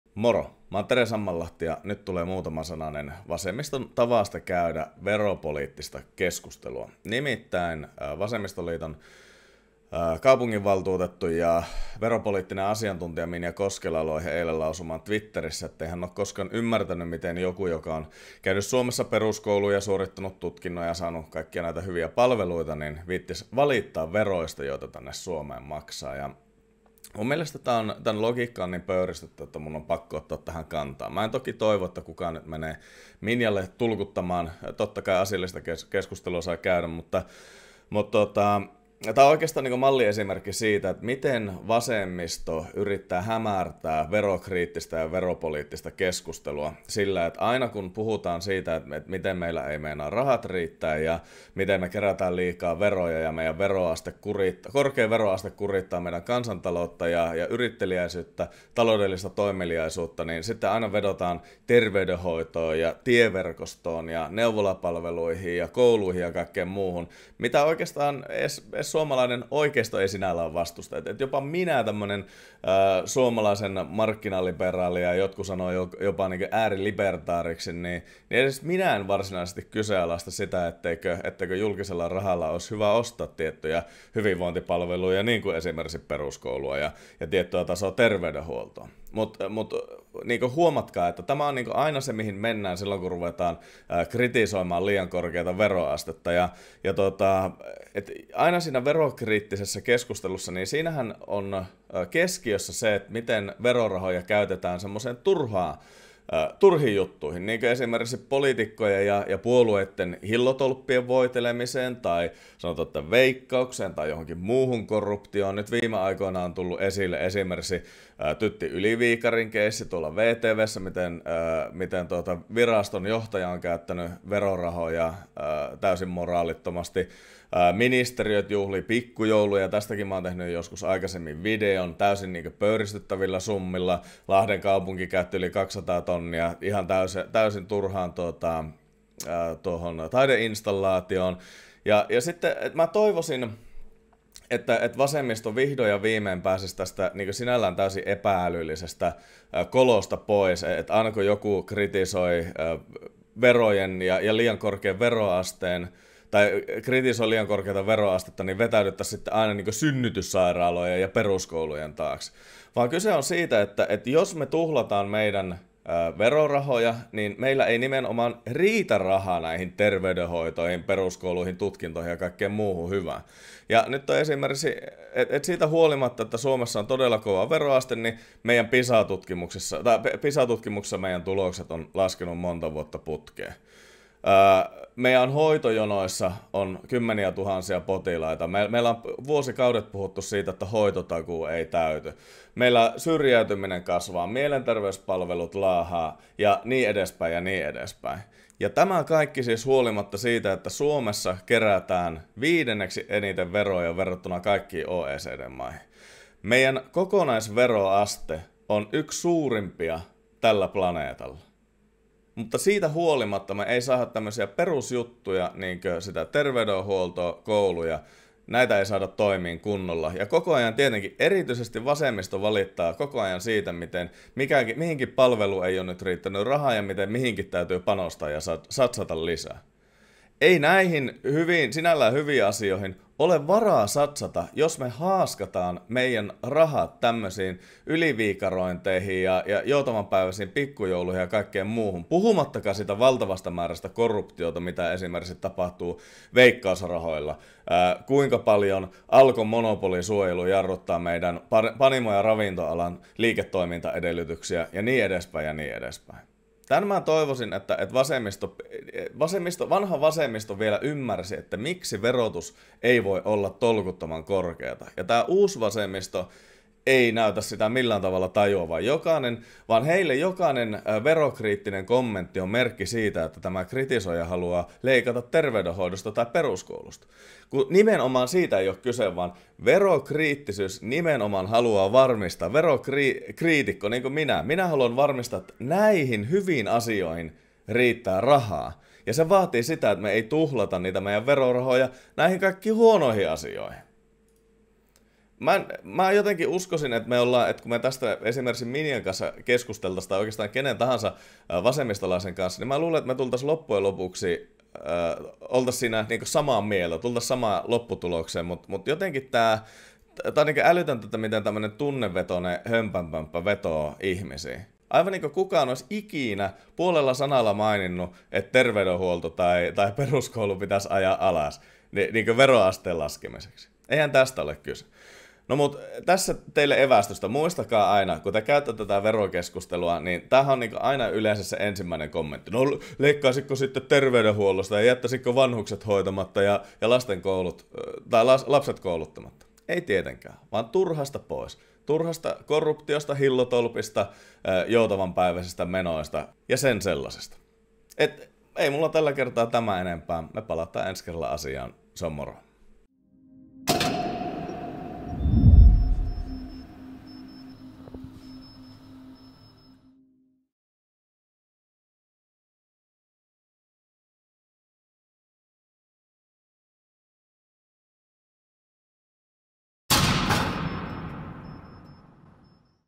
Kuka haluaa leikata peruskouluista? - Vasemmisto yrittää hämärtää verokriittistä keskustelua
Muista heittää peukut, tilata kanava ja tsekata mun muutkin somet. Tervetuloa mukaan keskusteluun!

Sisältö:
00:00 -